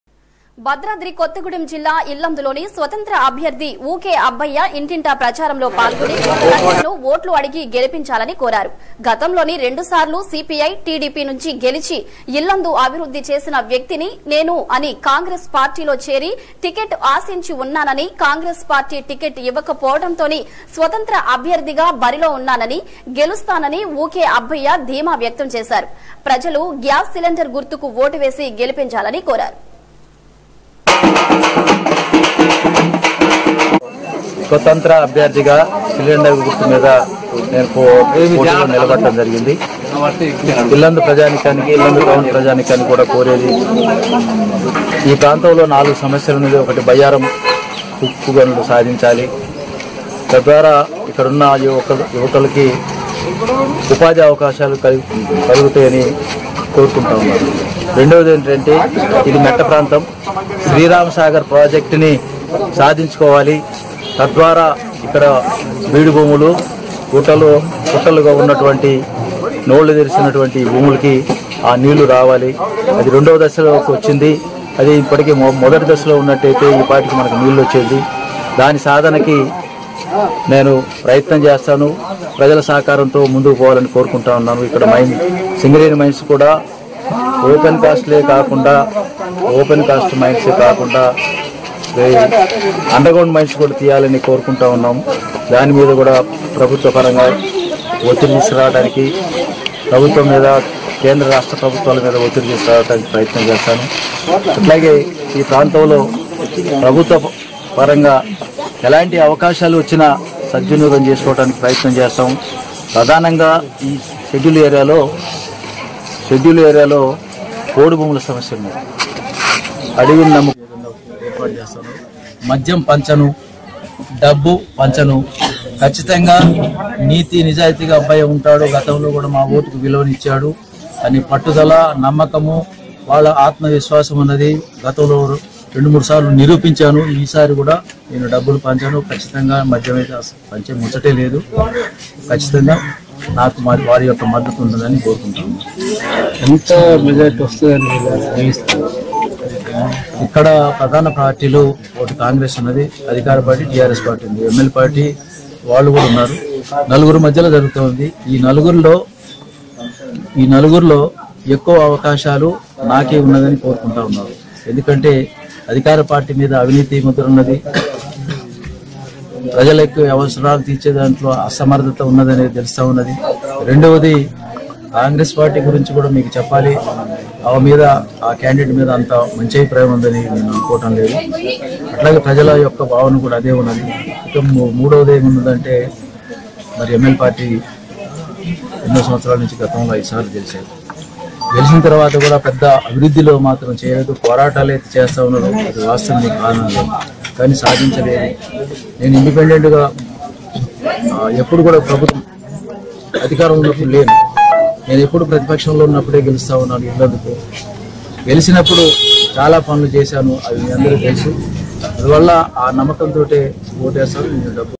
భద్రాద్రి కొత్తగూడెం జిల్లా ఇల్లందులో స్వతంత్ర అభ్యర్థి ఊకె అబ్బయ్య ఇంటింటి ప్రచారం
భద్రాద్రి కొత్తగూడెం జిల్లా ఇల్లందులో స్వతంత్ర అభ్యర్థి ఊకె అబ్బయ్య ఇంటింటి ప్రచారంలో పాల్గొని ప్రజలను ఓట్లు అడిగి గెలిపించాలని కోరారు. గతంలో రెండుసార్లు సీపీఐ, టీడీపీ నుంచి గెలిచి ఇ ల్లందు అభివృద్ధి చేసిన వ్యక్తిని నేను అని కాంగ్రెస్ పార్టీలో చేరి టిక్కెట్ ఆశించి ఉన్నానని కాంగ్రెస్ పార్టీ టిక్కెట్ ఇవ్వకపోవడంతో స్వతంత్ర అభ్యర్థి గా బరిలో ఉన్నానని గెలుస్తానని ఊకె అబ్బయ్య ధీమా వ్యక్తం చేశారు. ప్రజలు గ్యాస్ సిలిండర్ గుర్తుకు ఓటు వేసి గెలిపించాలని కోరారు..